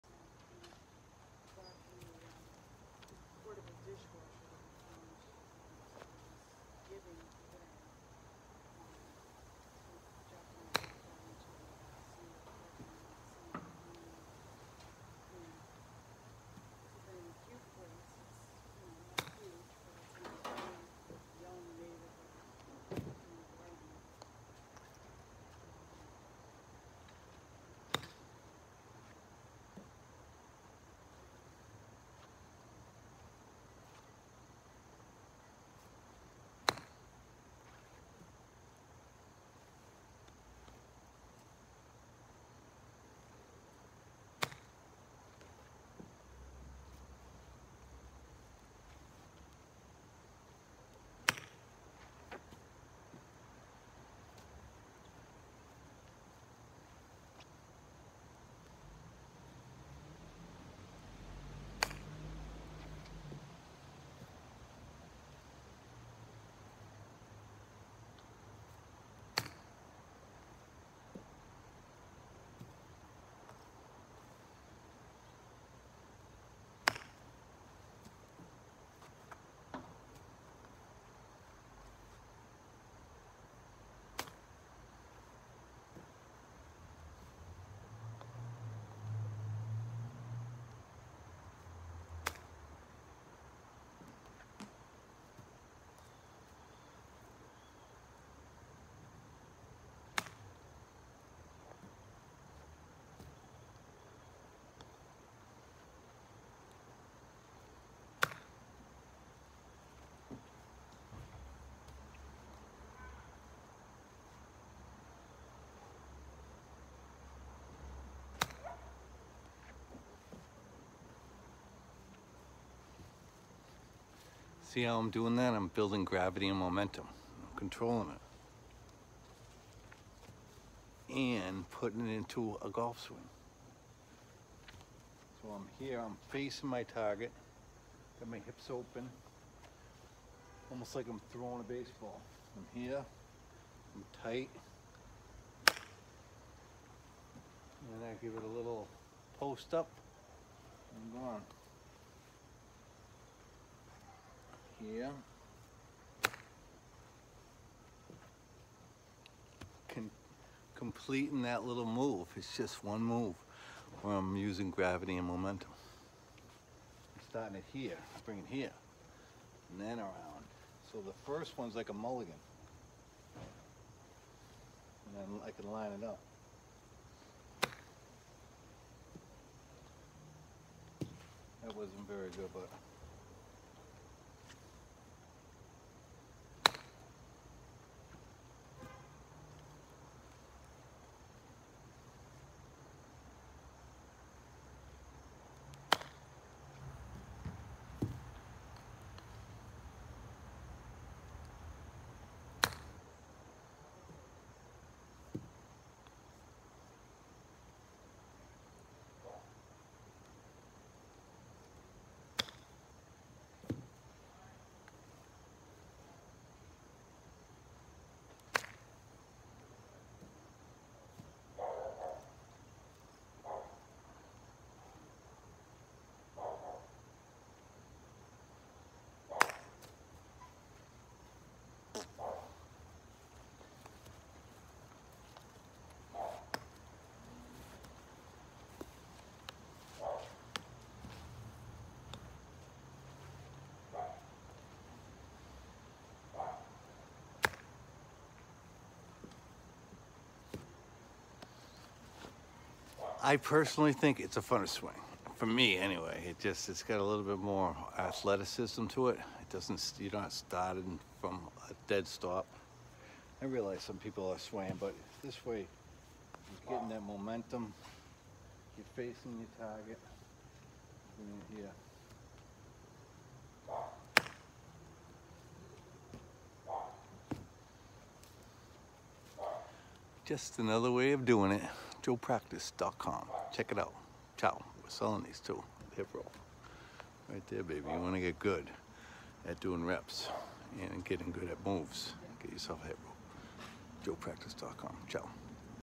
The Joe Show NEW way of swinging a golf club FOR SENIOR GOLFERS AND DISABLED PEOPLE & GIRLS & BOYS
This is a golf swing I invented that takes advantage of gravity and momentum, two of the greatest forces of earth. JoePractice.com
 Good Luck
Joe Practice Sports please tell your friends to check us out. Thanks and good luck with your swinging a golf club and hitting the golf ball straight down the middle.
I'm using a 52 degree wedge made by Wilson. Love it, my oldest son bought it for me. Works great. Remember it's not the arrow it's the archer